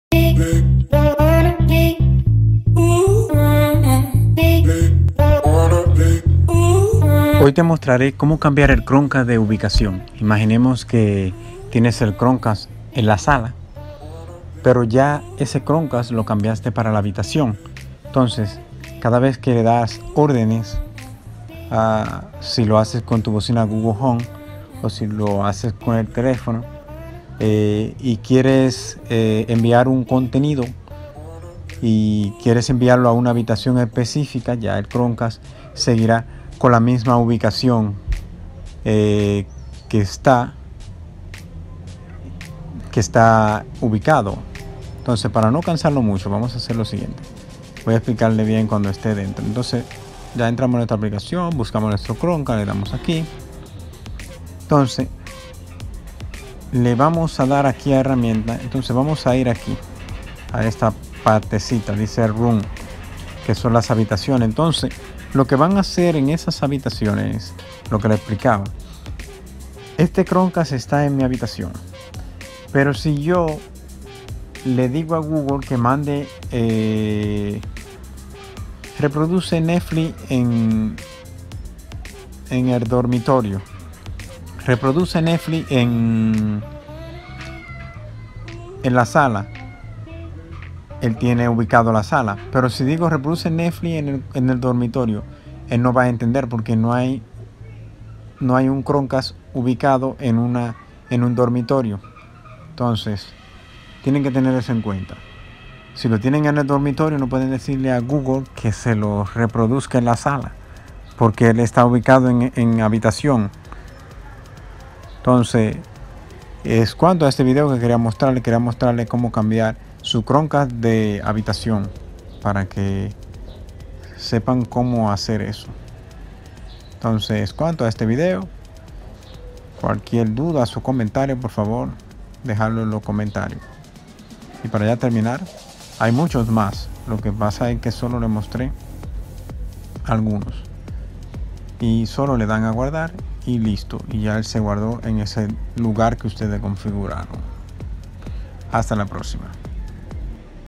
Como cambiar el Chromecast Ultra la Ubicación
Esto es cambiar los lugares donde ya lo tienes configurado. Ejemplo
lo usas en la sala,  y lo mueves a la habitación, va a funcionar pero si tienes un asistente de google y le dices reproducir netflix en la habitación.  Ya sabes que si tenias puesta la configuración en la sala no reproduce nada.